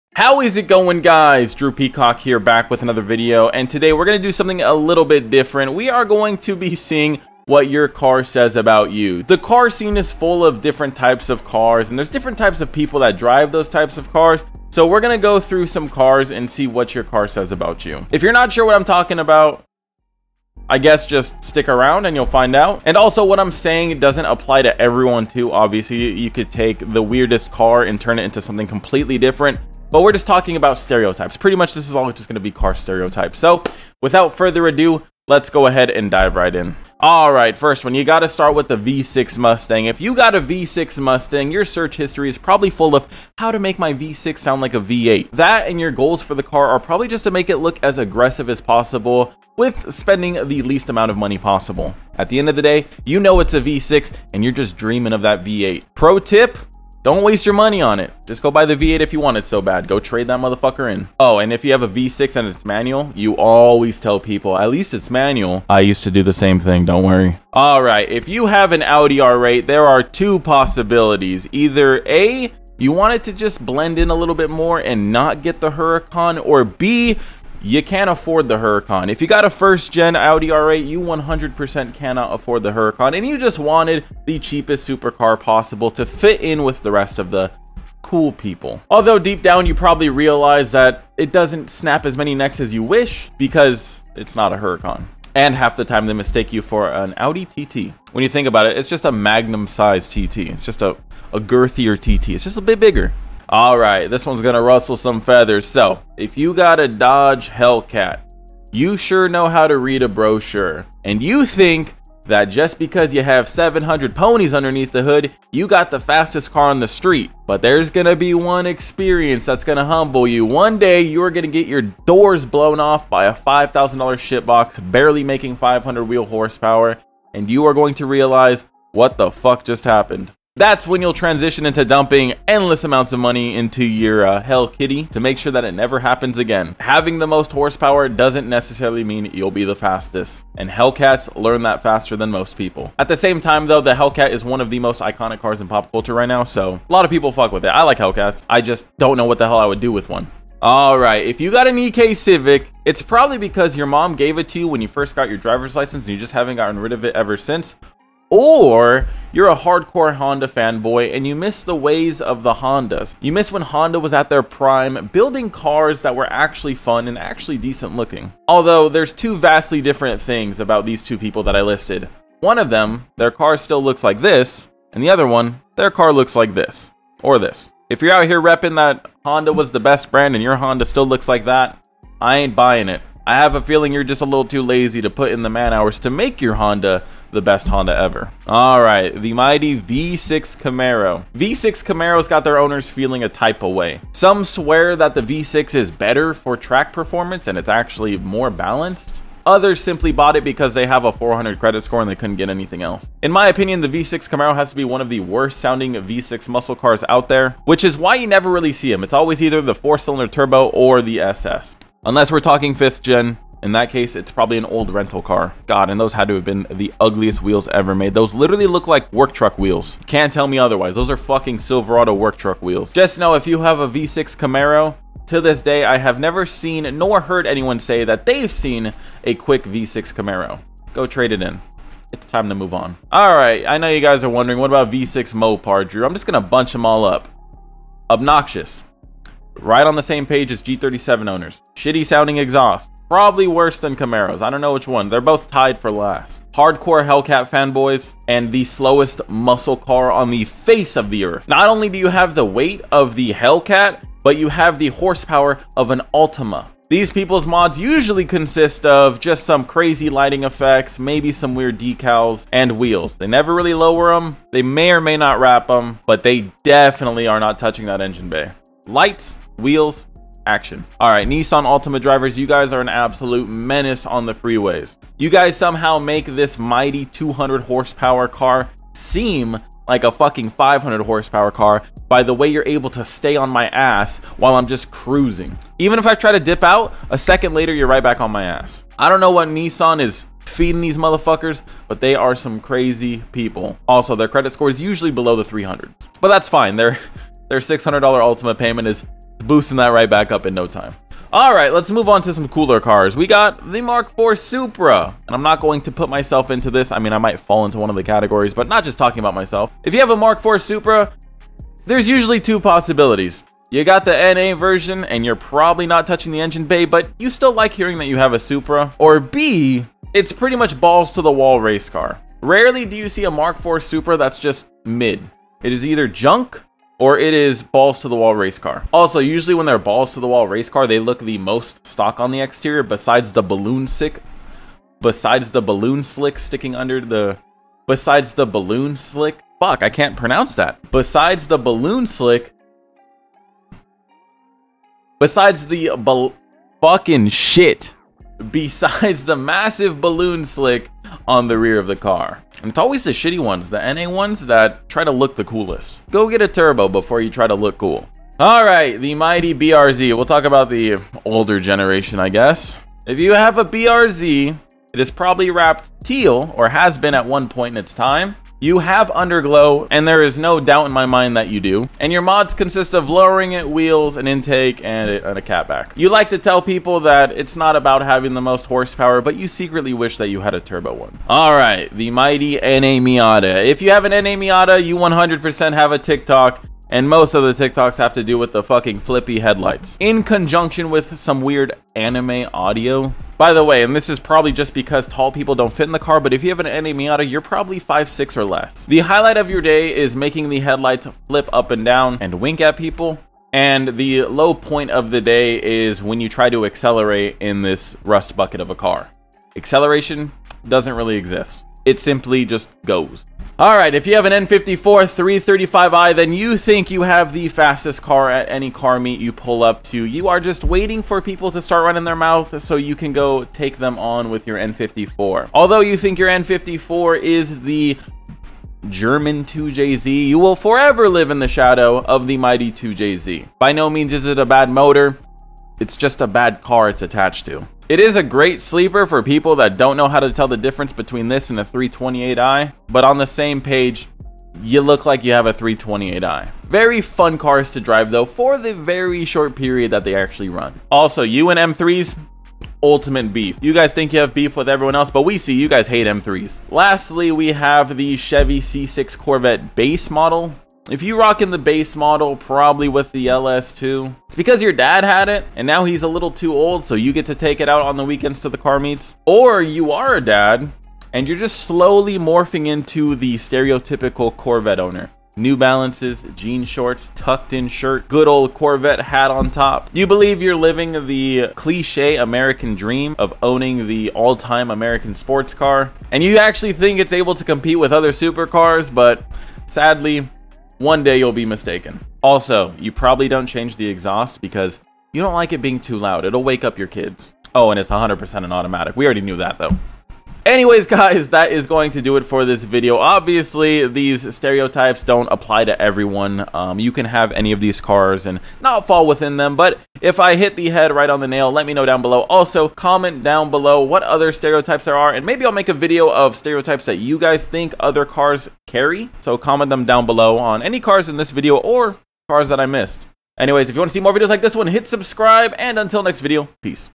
What Your Car Says About You!!!
Do you believe in car stereotypes or have heard of any in the past ? Here’s a handful of some and let’s see if you guys are guilty of any!